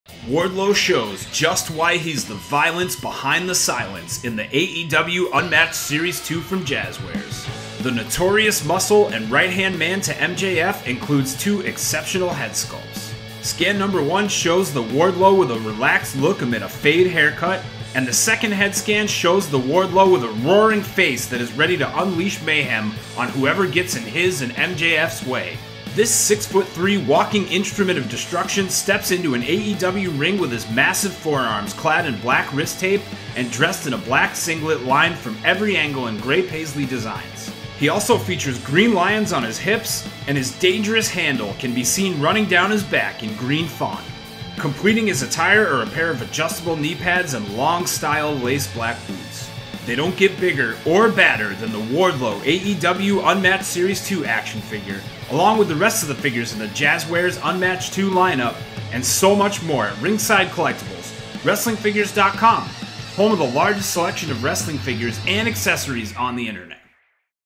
AEW Figure Insider: Wardlow - Jazwares AEW Unmatched 2 Wrestling Action Figure!
Winter isn't just coming...it has arrived with an inside look at the Wardlow AEW Unmatched 2 wrestling figure from Jazwares!